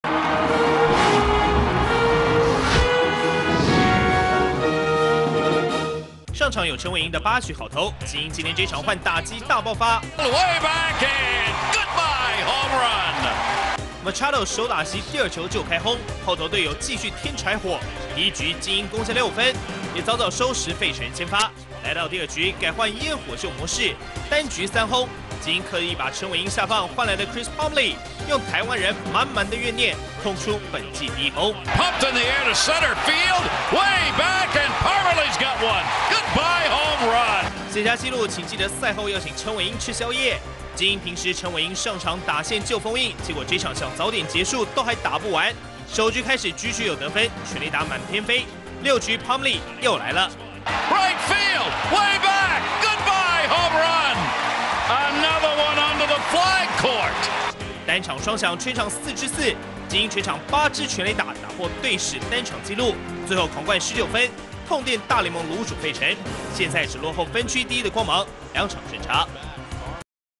20150617MLB金鶯單場8轟 終場19:3重創費城人
Voice of:Gary Thorne